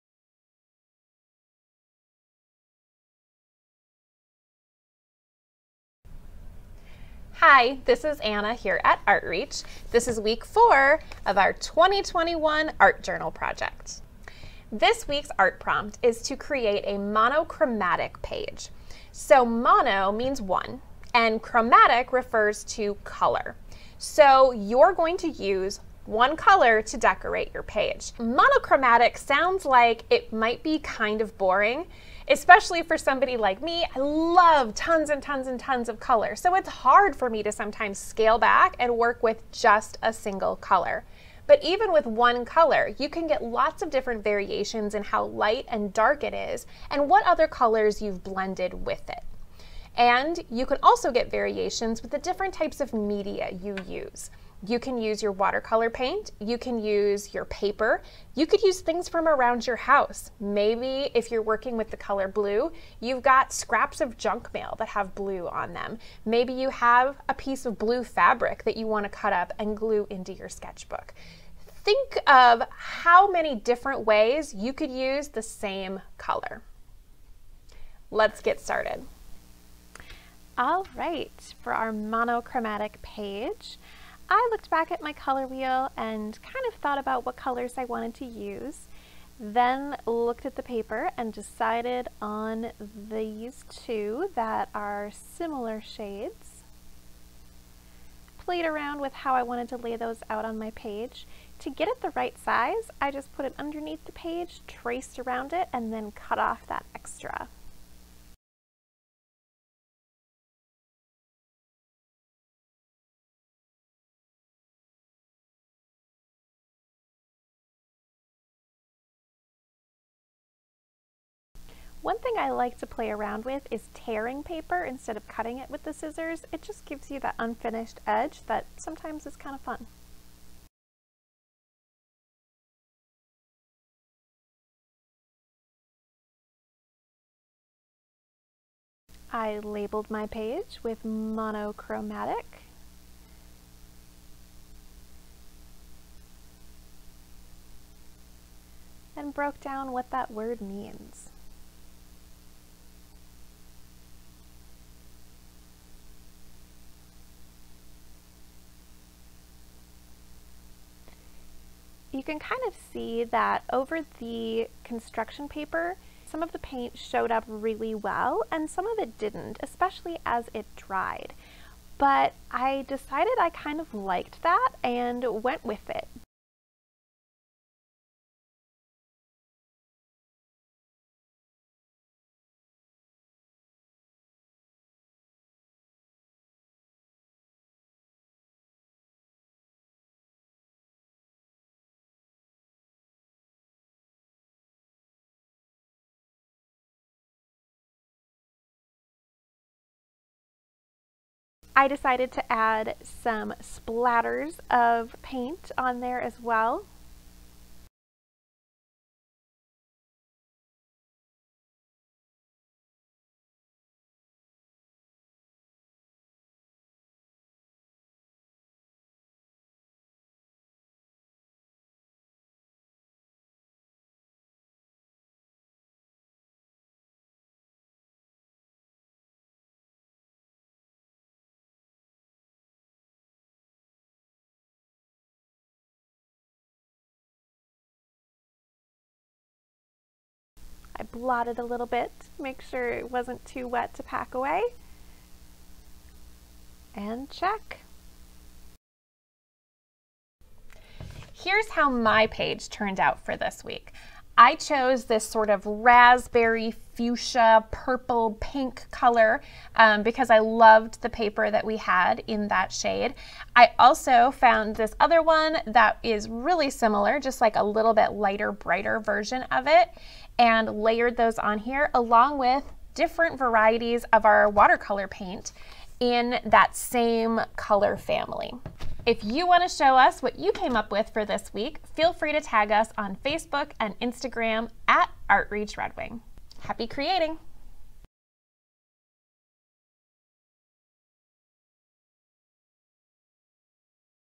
2021 Art Journal Week 4 Monochromatic
Mono = one, Chromatic = color
How many ways can you use just one color?